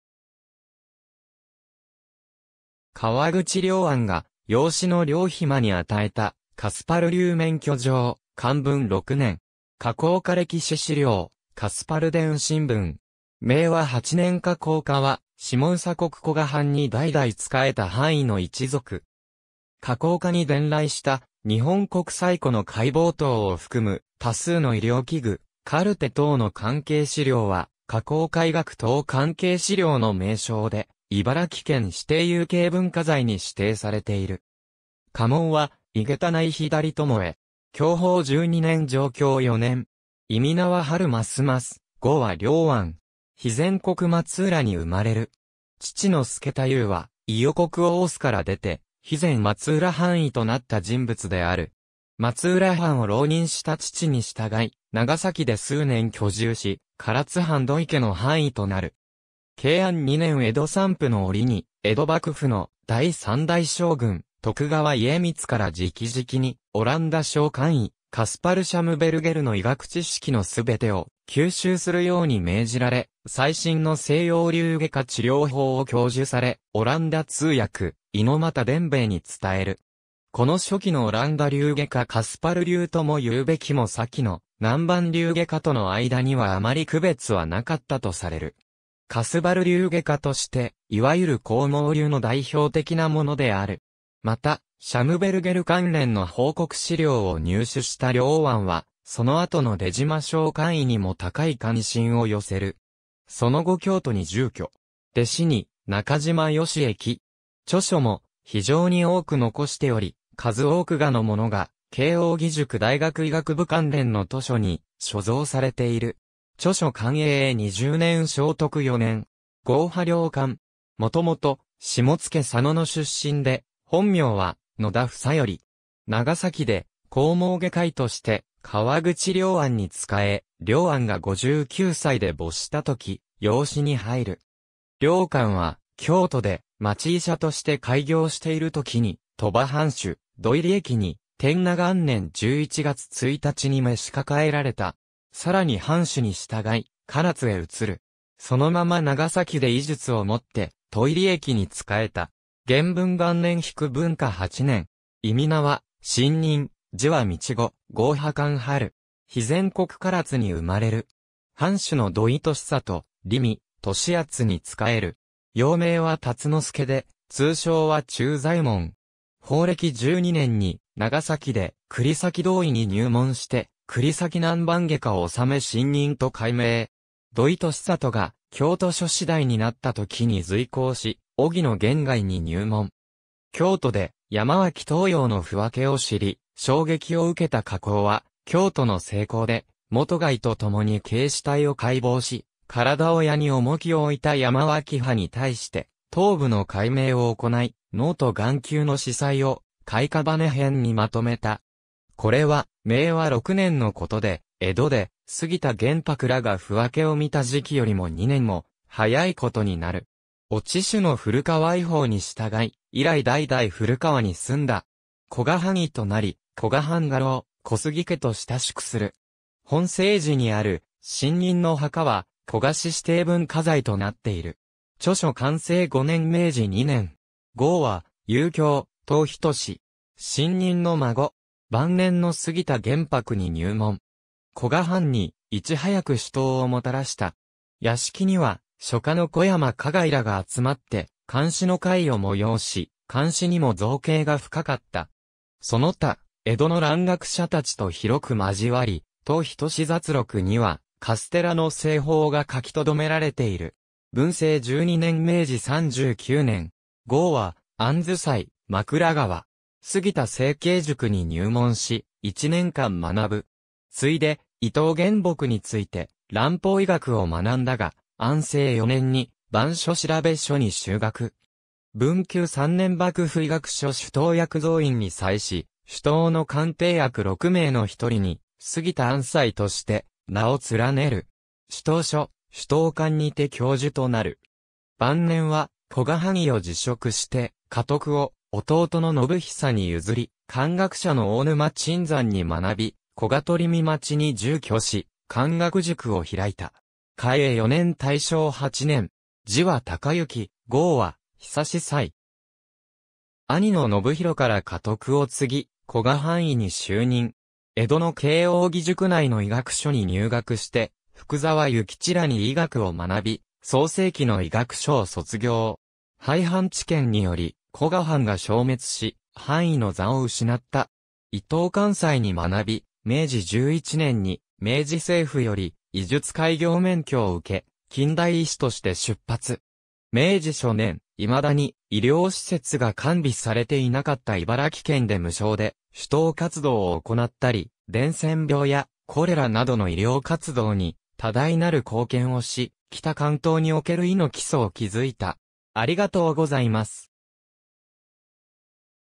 河口家
河口良庵が養子の了閑に与えた「カスパル流免許状」寛文6年(1666年)。
河口家歴史資料(古河歴史博物館保管) カスパル伝神文。
明和8(1771)年（個人蔵） 河口家（かわぐちけ）は、下総国古河藩（現在の茨城県古河市）に代々仕えた藩医の一族。
河口家に伝来した日本国最古の解剖刀を含む多数の医療器具、カルテ等の関係資料は、「河口家医学等関係資料」の名称で茨城県指定有形文化財（歴史資料）に指定されている（2007年11月16日指定）。
家紋は井桁内左巴。
享保12年（1629年） - 貞享4年（1687年）。
諱は春益、号は良庵。
肥前国松浦に生まれる。
父の佐太夫は、伊予国大洲から出て肥前松浦藩医となった人物である。
松浦藩を浪人した父に従い、長崎で数年居住し、唐津藩土井家の藩医となる。
慶安2年（1649年）江戸参府の折に江戸幕府の第3代将軍・徳川家光から直々に、オランダ商館医・カスパル・シャムベルゲルの医学知識の全てを吸収するように命じられ、最新の西洋流外科治療法（カスパル流外科）を教授され、オランダ通訳・猪股伝兵衛に伝える。
この初期のオランダ流外科カスパル流ともいうべきもさきの南蛮流外科との間には余り区別はなかったとされる。
カスバル流外科としていわゆる紅毛流の代表的なものである。
また、シャムベルゲル関連の報告資料を入手した良庵は、その後の出島商館医にも高い関心を寄せる。
その後京都に住居。
弟子に中嶋善益。
著書も非常に多く残しており、数多くがのものが慶應義塾大学医学部関連の図書に所蔵されている。
著書 寛永20年（1644年） - 正徳4年（1714年）。
号は了閑。
もともと下野佐野の出身で、本名は野田房頼。
長崎で紅毛外科医として河口良庵に仕え、良庵が五九歳で没した時、養子に入る。
良閑は京都で町医者として開業している時に、鳥羽藩主・土井利益に天和元年11月1日に召し抱えられた。
さらに藩主に随い唐津へ移る。
そのまま長崎で医術をもって土井利益に仕えた。
元文元年（1736年） - 文化8年（1811年）。
諱は信任、字は道五、号は閑春。
肥前国唐津（現・佐賀県唐津市）に生まれる。
藩主の土井利里、利見、利厚に仕える。
幼名は辰之助で通称は忠左衛門。
宝暦12年に長崎で栗崎道意に入門して栗崎南蛮外科を修め信任と改名。
土井利里が京都所司代になった時に随行し、荻野元凱に入門。
京都で山脇東洋の腑分を知り、衝撃を受けた河口は、京都の西郊で元凱とともに刑死体を解剖し、 体親に重きを置いた山脇派に対して、頭部の解明を行い、脳と眼球の子細を『解屍編』に纏めた。
これは明和6年（1769年）のことで、江戸で杉田玄白らが腑分を見た時期よりも2年も早いことになる。
落主の古河移封に随い、以来代々古河に住んだ。
古河藩医となり、古河藩家老・小杉家と親しくする。
本成寺にある信任の墓は、古河市指定文化財となっている。
著書 寛政5年（1793年） - 明治2年（1869年）。
号は祐卿、陶斎。
信任の孫。
晩年の杉田玄白に入門。
古河藩にいち早く種痘をもたらした。
屋敷には書家の小山霞外らが集まって漢詩の会を催し、漢詩にも造詣が深かった。
その他、江戸の蘭学者達と広く交わり、『陶斎雑録』には「カステラの製法」が書き留められている。
文政12年（1829年） - 明治39年（1906年）。
号は杏斎、枕河。
杉田成卿塾に入門し、1年間学ぶ。
次いで伊東玄朴について蘭方医学を学んだが、安政4年（1857年）に蕃書調所に修学。
文久3年（1863年）幕府医学所種痘役増員に際し、種痘の鑑定役六名の一人に「杉田杏斎」として名を連ねる。
種痘所・種痘館（東京大学医学部の前身）にて教授となる。
晩年は古河藩医を辞職して家督を弟の信久に譲り、漢学者の大沼枕山に学び、古河鳥見町に住居し漢学塾を開いた。
嘉永4年（1851年） - 大正8年（1919年）。
字は敬之、号は久斎。
兄の信寛から家督を継ぎ、古河藩医に就任。
江戸の慶應義塾内の医学所（慶應義塾医学所）に入学して福澤諭吉らに医学を学び、創成期の医学所を卒業。
廃藩置県により古河藩が消滅し、藩医の座を失った。
伊東貫斎に学び、明治11年（1878年）に明治政府より医術開業免許を受け、近代医師として出発。
明治初年、未だに医療施設が完備されていなかった茨...